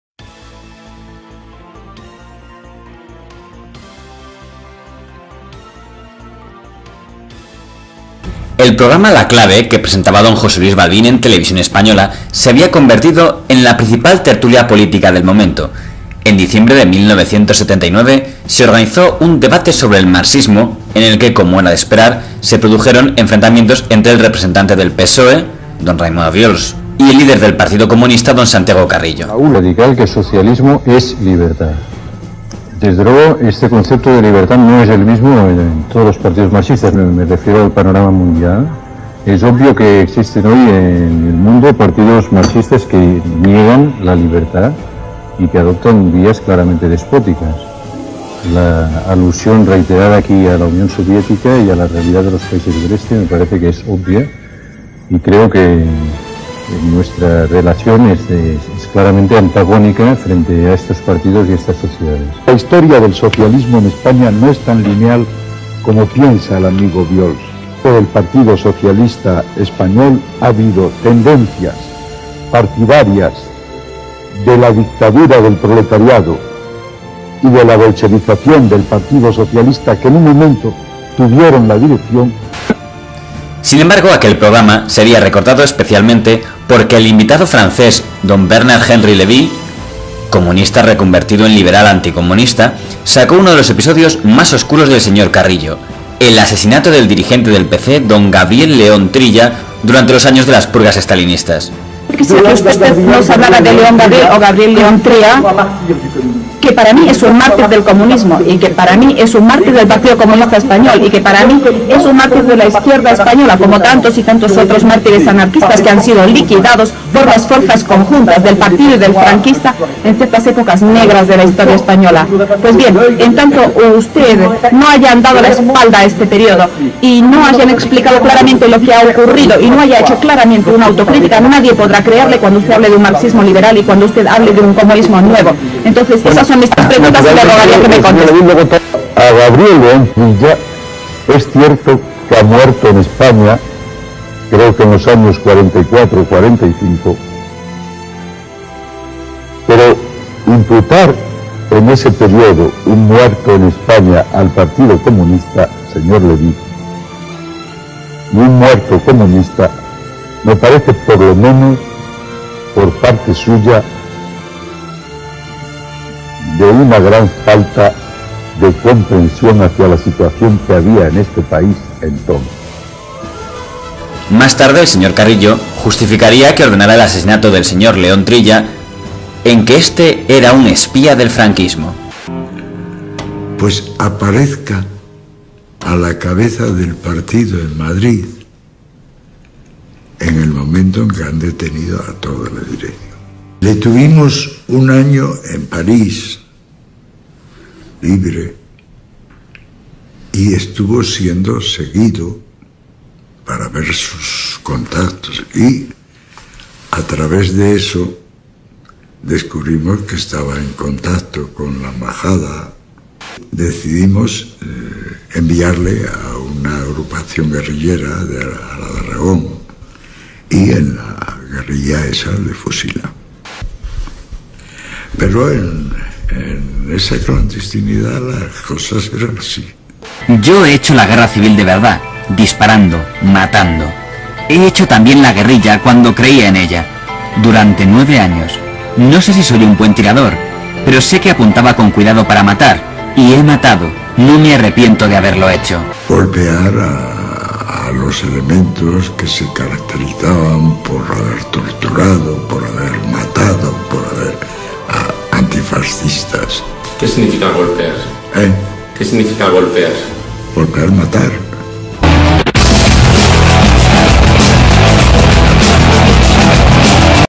Santiago Carrillo (PCE) acusado de asesinar a Gabriel León Trilla - 1979
Henri Levy acusó ante las cámaras de TVE a Santiago Carrillo de haber ordenado el asesinato de Gabriel León Trilla en los años de clandestinidad (Carrillo lo acusó de ser 'agente doble'). El caso había sido hecho público por Jorge Semprún.